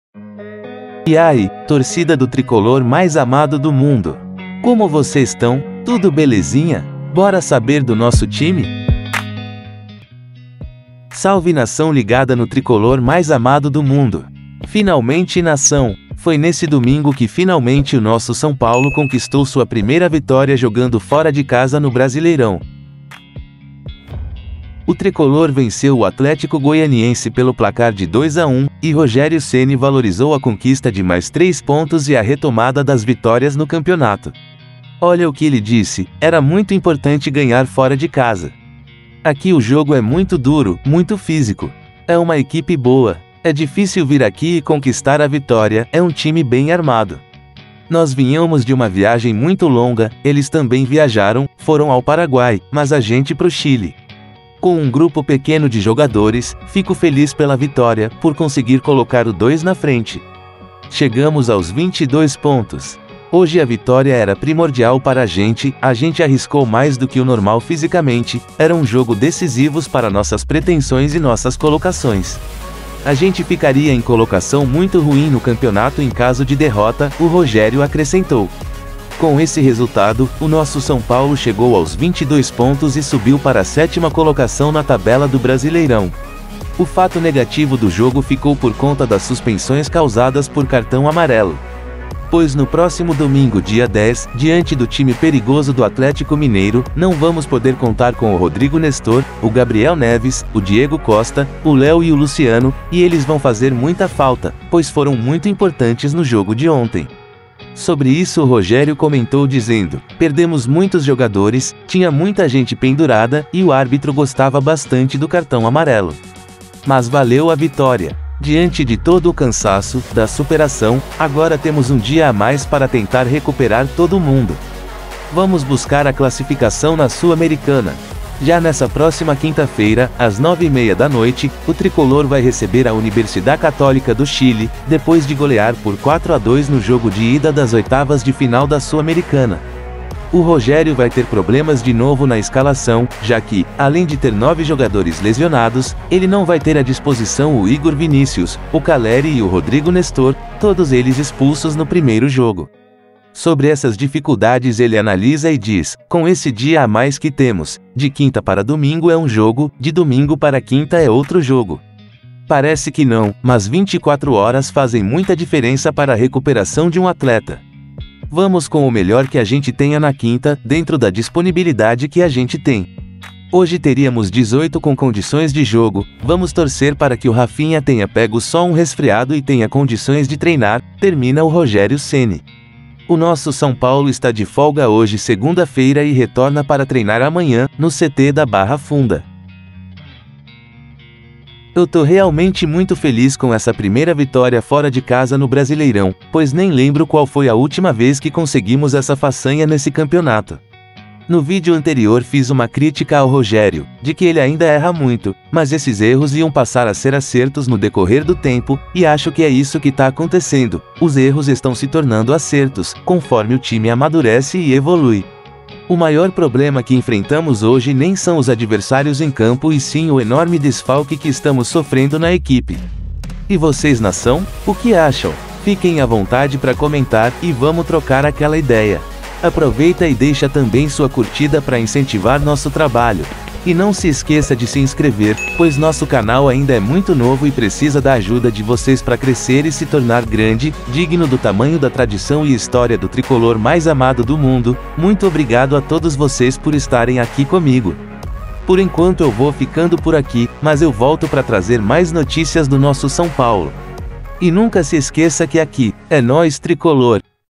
ATENÇÃO | Rogério Ceni comemora vitória mas lamenta mais desfalques
Rogério Ceni comemora vitória mas lamenta mais desfalques

e Compre a melhor tv box do mercado e nunca mais perca os jogos do nosso tricolor e muito mais, aproveite nossa oferta exclusiva! ÚLTIMAS UNIDADES!

Finalmente nação! Foi nesse domingo que finalmente o nosso sãopaulo Paulo conquistou sua primeira vitória jogando fora de casa no Brasileirão. O tricolor Venceu o Atlético-GO pelo placar de 2 a 1, e rogérioceni valorizou a conquista de mais três pontos e a retomada das vitórias no Campeonato.
Olha o que ele disse:
“Era muito importante ganhar fora de casa. Aqui o jogo é muito duro, muito físico. É uma equipe boa. É difícil vir aqui e conquistar a vitória, é um time bem armado. Nós vínhamos de uma viagem muito longa, eles também viajaram, foram ao Paraguai, mas a gente pro Chile. Com um grupo pequeno de jogadores, fico feliz pela vitória, por conseguir colocar o dois na frente. Chegamos aos 22 pontos”.
“Hoje a vitória era primordial para a gente, a gente arriscou mais do que o normal fisicamente, era um jogo decisivo para nossas pretensões e nossas colocações. A gente ficaria em colocação muito ruim no campeonato em caso de derrota”, o Rogério acrescentou.
Com esse resultado, o nosso São Paulo chegou aos 22 pontos e subiu para a sétima colocação na tabela do Brasileirão. O fato negativo do jogo ficou por conta das suspensões causadas por cartão amarelo. Pois no próximo domingo dia 10, diante do time perigoso do Atlético-MG, não vamos poder contar com o Rodrigo Nestor, o Gabriel Neves, o Diego Costa, o Léo e o Luciano, e eles vão fazer muita falta, pois foram muito importantes no jogo de ontem.
Sobre isso o Rogério comentou dizendo:
“Perdemos muitos jogadores, tinha muita gente pendurada, e o árbitro gostava bastante do cartão amarelo. Mas valeu a vitória. Diante de todo o cansaço, da superação, agora temos um dia a mais para tentar recuperar todo mundo. Vamos buscar a classificação na Sul-Americana”.

Já nessa próxima quinta-feira, às 21h30, o Tricolor vai receber a Universidad Católica, do Chile, depois de golear por 4 a 2 no jogo de ida das oitavas de final da Sul-Americana.

O Rogério vai ter problemas de novo na escalação, já que, além de ter nove jogadores lesionados, ele não vai ter à disposição o Igor Vinicius, o Calleri e o Rodrigo Nestor, todos eles expulsos no primeiro jogo.
Sobre essas dificuldades ele analisa e diz:

“Com esse dia a mais que temos... de quinta para domingo é um jogo, de domingo para quinta é outro jogo. Parece que não, mas 24 horas fazem muita diferença para a recuperação de um atleta. Vamos com o melhor que a gente tenha na quinta, dentro da disponibilidade que a gente tem. Hoje teríamos 18 com condições de jogo, vamos torcer para que o Rafinha tenha pego só um resfriado e tenha condições de treinar”, termina o Rogério Ceni.

O nosso São Paulo está de folga hoje segunda-feira e retorna para treinar amanhã, no CT da Barra Funda.

Crédito Imagens (Fotos): Marcos Ribolli, spfc.net, Rubens Chiri / saopaulofc.net / São Paulo FC